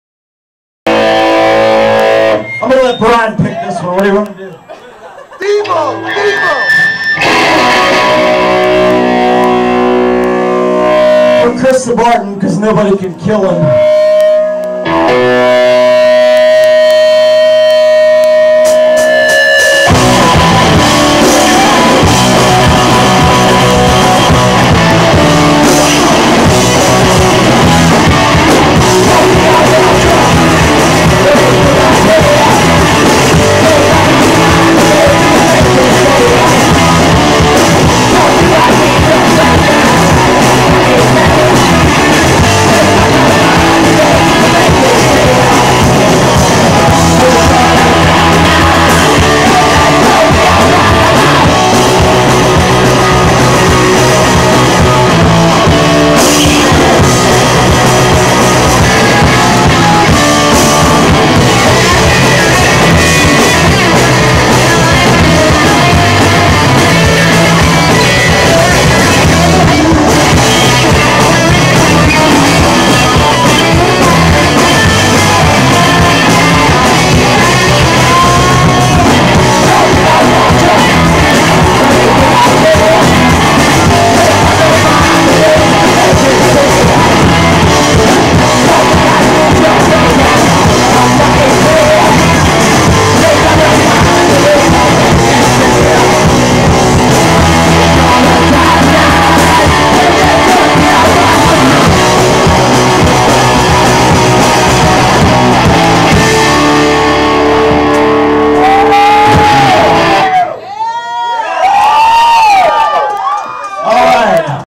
Mud City Manglers @ smiling moose
I always thought this band had a cool new bomb turks vibe. I dunno what the song is called.